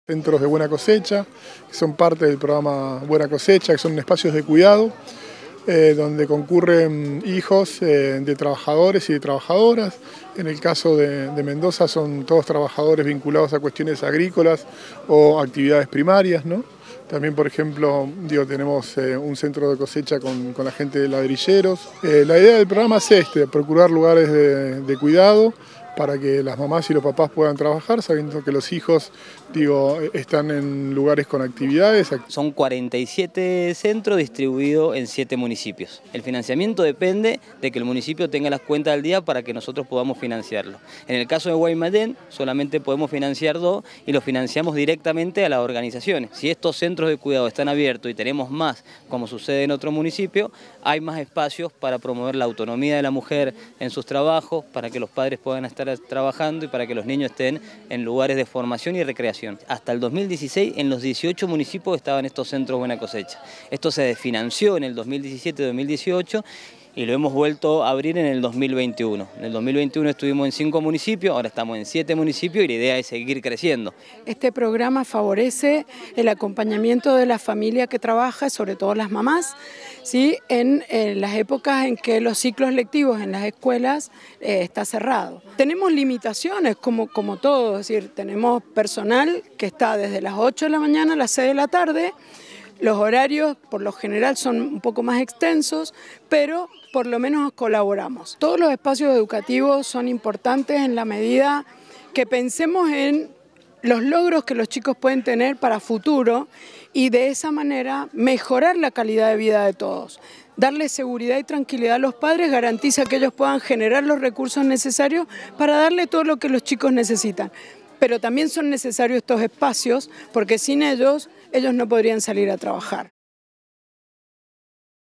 Gran Mendoza: Jardines Buena Cosecha contra el trabajo infantil
El Programa Buena Cosecha es una herramienta para erradicar el trabajo infantil. Se trata de espacios de cuidado y contención para hijas e hijos de trabajadores/as del ámbito rural. Uno de ellos es el Jardín Maternal Los Angelitos, ubicado en Guaymallén.